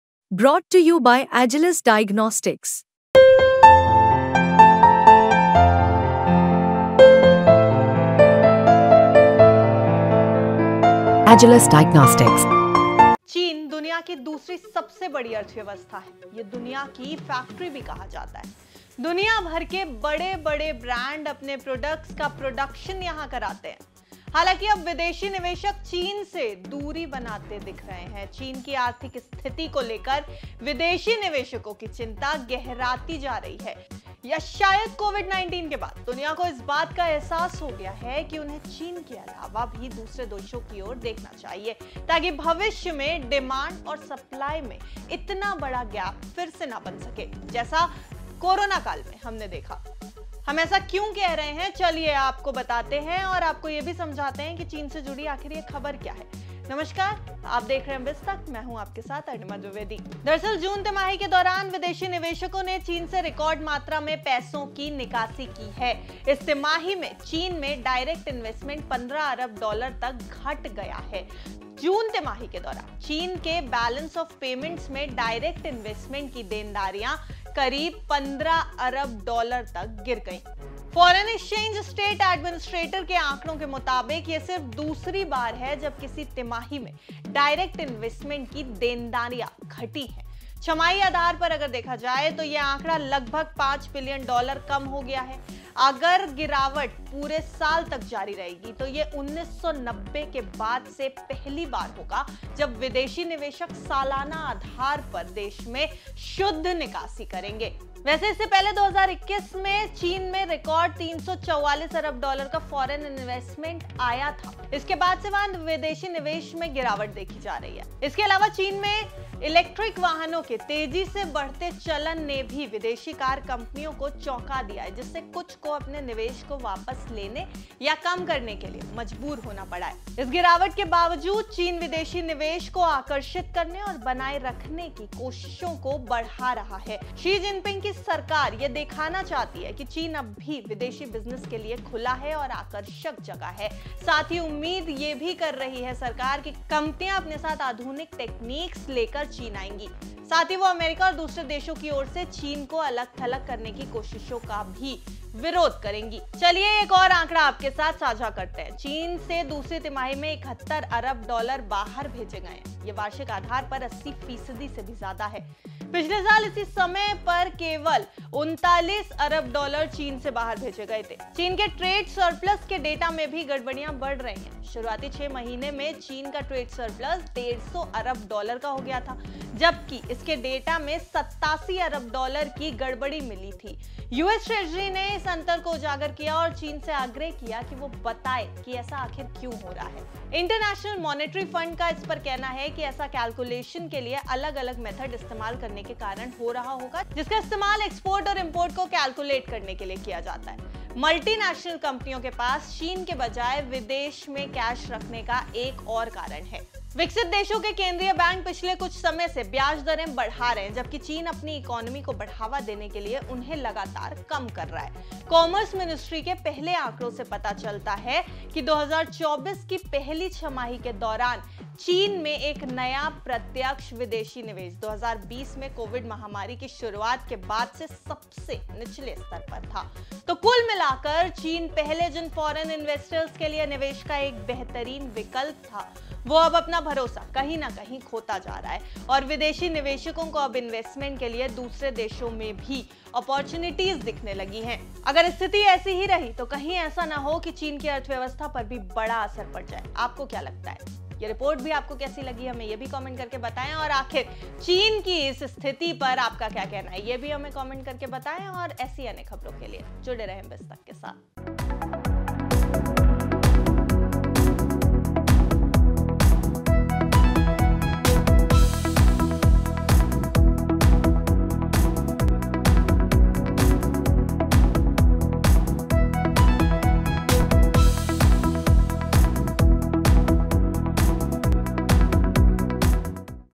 China से उठ गया Foreign Investors का भरोसा, अब क्या होगा? | BIZ Tak
China’s economic slowdown has been worrying investors and increasing pessimism, forcing them to pull out their money from the nation. The recent shocking investment numbers are proof to it which shows foreign investors have pulled out a record $15 billion from China in just the last quarter. Watch this video to know more details.

BizTak Premium ज्वाइन करने के लिए क्लिक करें .